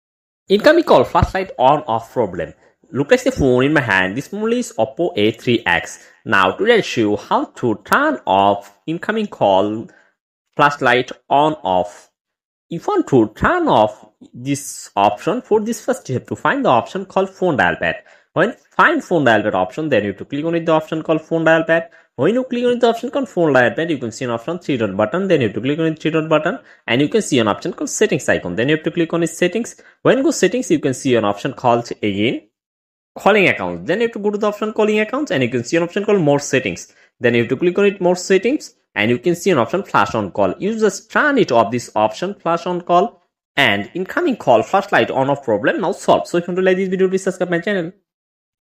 Oppo A3x incoming call Flash Light on off problem
When incoming call come to the phone your phone flashlight on off problem. now today i will show you how to solve this problem incoming call flashlight on problem in oppo a3x phone.
if you want to solve this problem incoming call flashlight on problem find the option called phone dial pad and when find phone dial pad option than you have to click on this option and see flash on call option than you have to click on this option and you just turn off this option and solve your phone incoming call flashlight on off problem in oppo a3x.